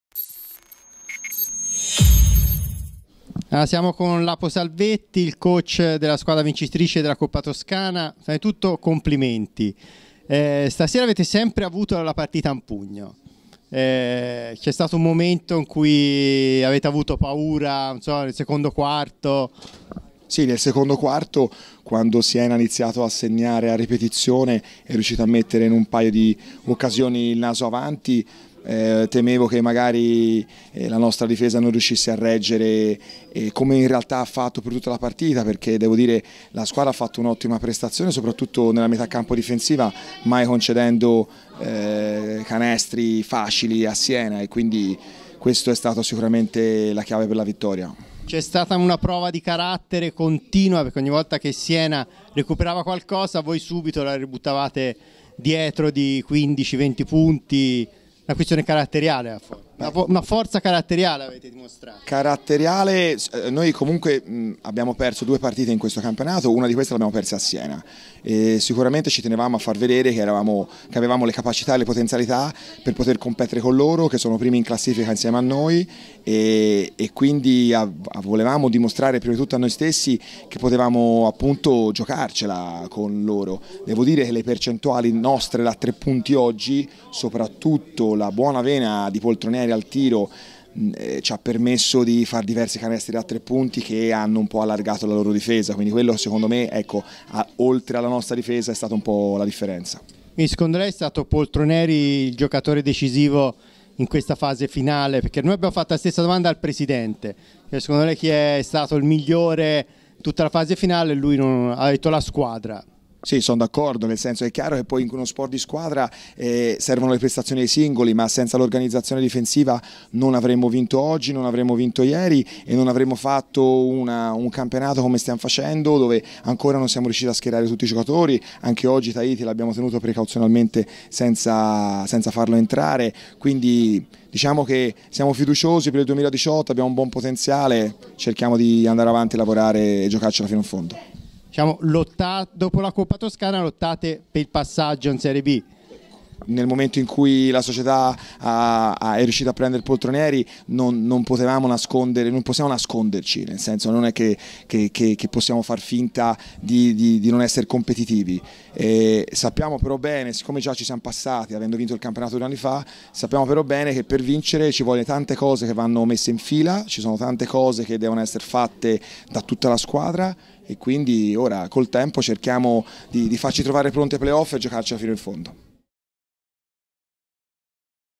Salvetti dopo la vittoria della Coppa Toscana 2017
Intervista di Riccardo Burgalassi